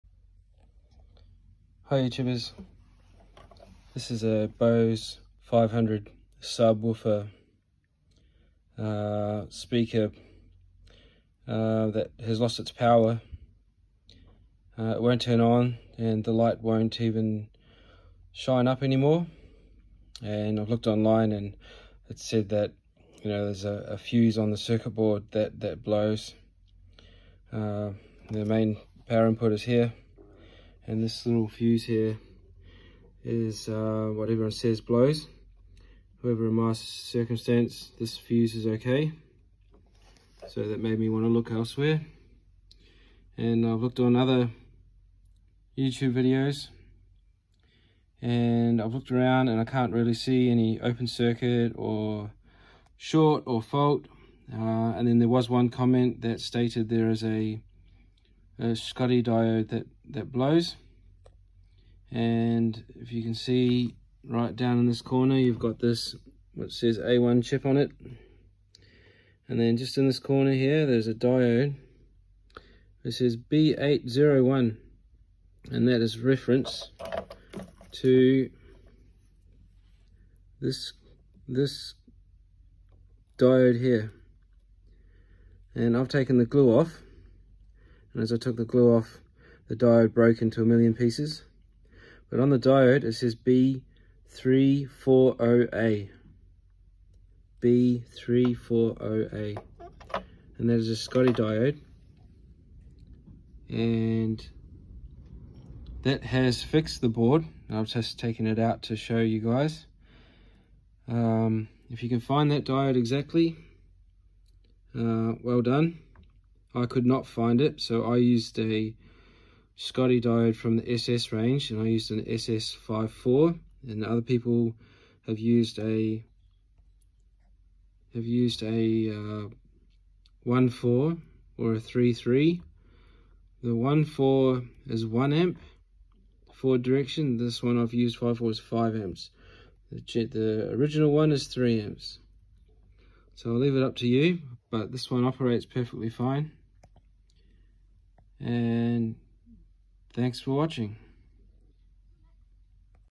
Bose 500 base no power stopped working
Bose 500 base box has no power, the schotty diode is a 3mah forward current diode. I use an SS54 schotty diode to replace.